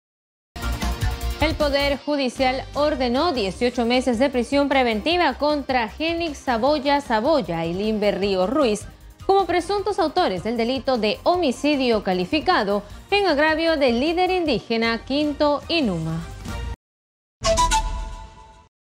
🔴🔵PJ ordena prisión preventiva contra sujetos que habrían asesinado a líder indigena Quinto Inuma
El Poder Judicial (PJ) ordenó 18 meses de prisión preventiva contra sujetos que habrían asesinado a líder indigena Quinto Inuma.

Exitosa Perú
"Exitosa: La voz que integra al Perú"

Corporación Universal